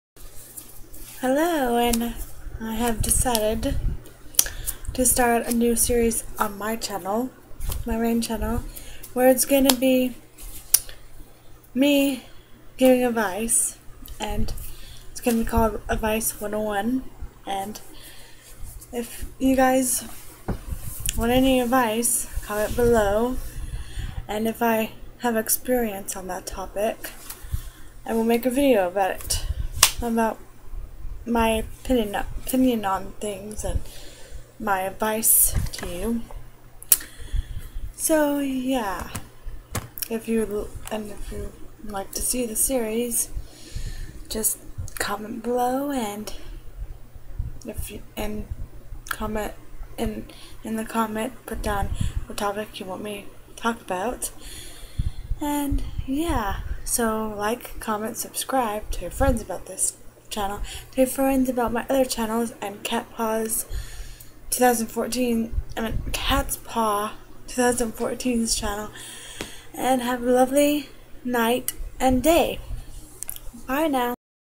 advice 101: intro
a new series i will be starting if i get any topics suggested or i will think of topics to give advice on that i have experienced